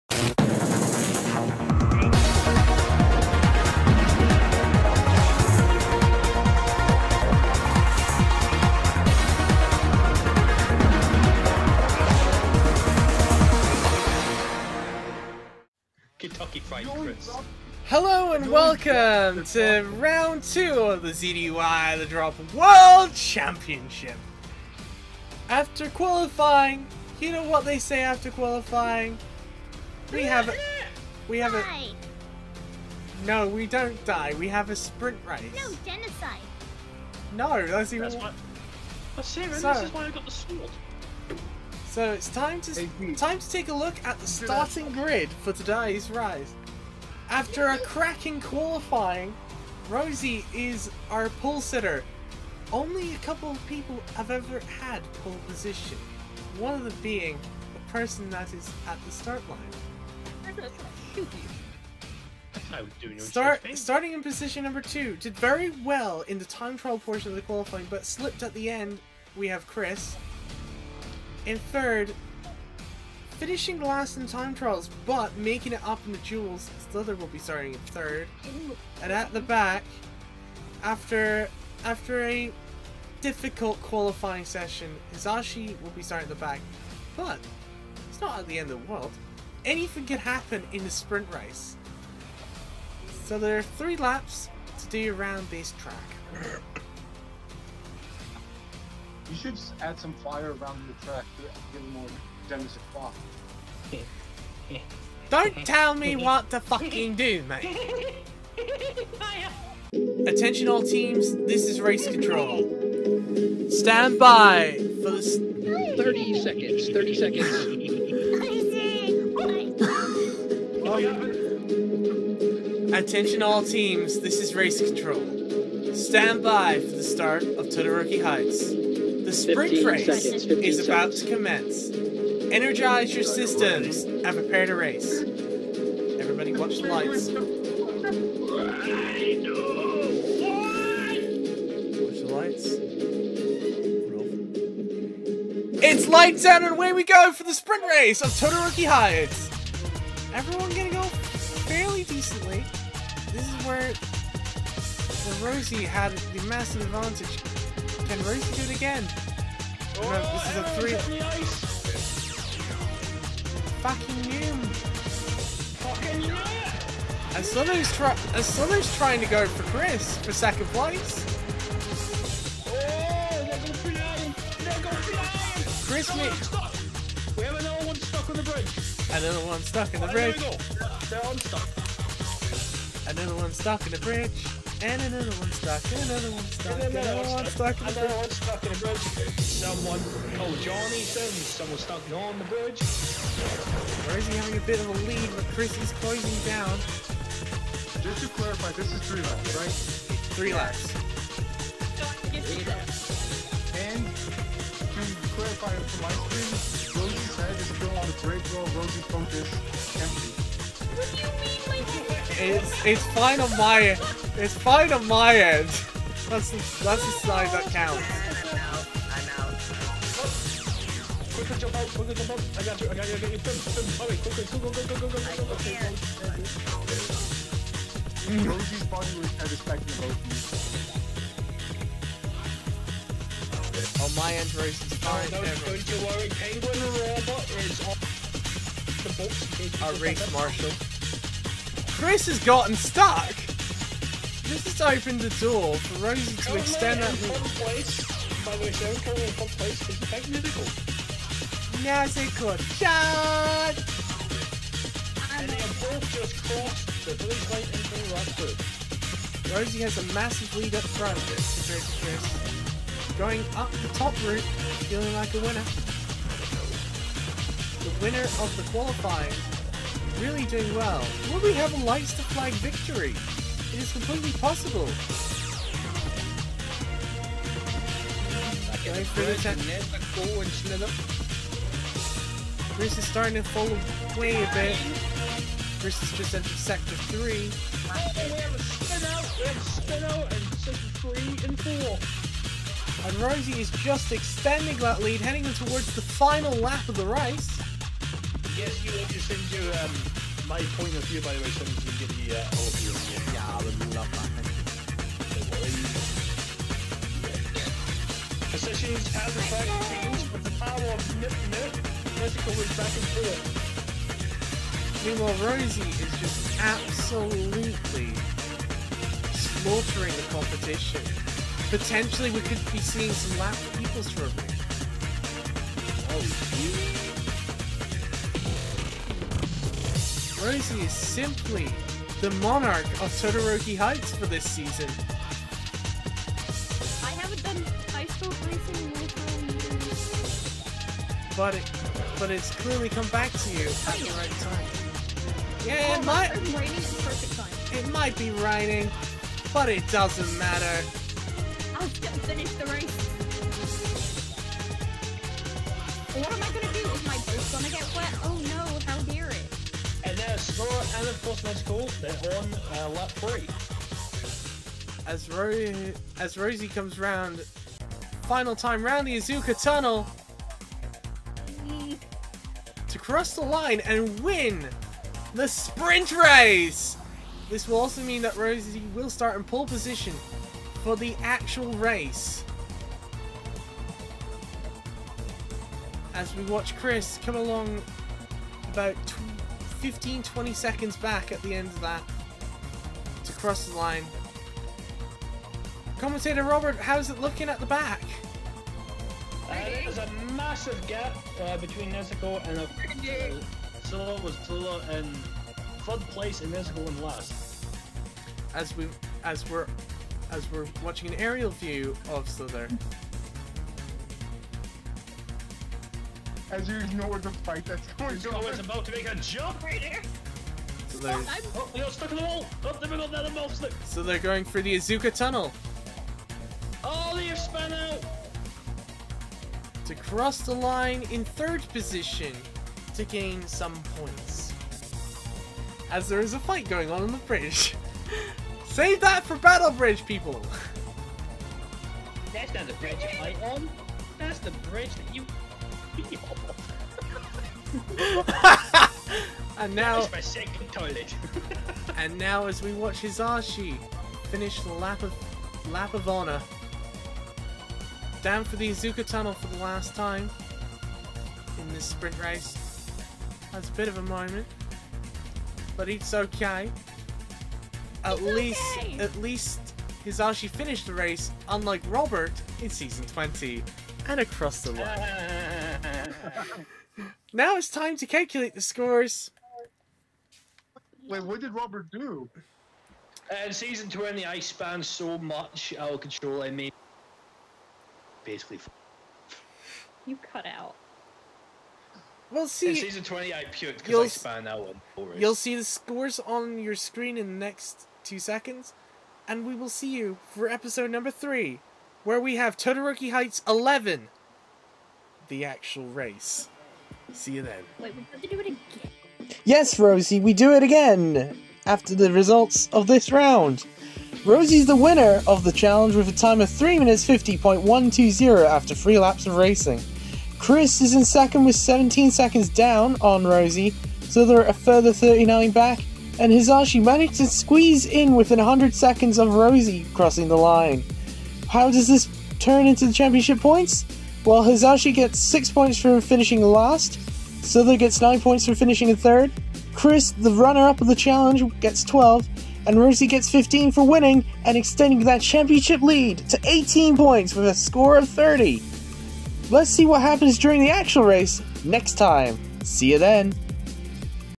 The Drop: Season 28 - Duel Destinies | Round 2: Todoroki Heights SPRINT Race
Welcome back to another episode of the 28th Season of the ZDY The Drop World Championship!

In tonight's episode, we have a three lap sprint race around the Todoroki Heights XI track! Who will win the sprint race and start on the @RDF GAMES+ Pole Position for Round 3?

Hosts: @CielAmamiya & @RDFRetro

Contestants:
Chris - chris_pedro_ (TikTok)
Hizashi - hizashixyamada0837 (TikTok)
Rosey - @burnet_rose7420 (YouTube/Twitch)
Slyther - slythernyx (TikTok)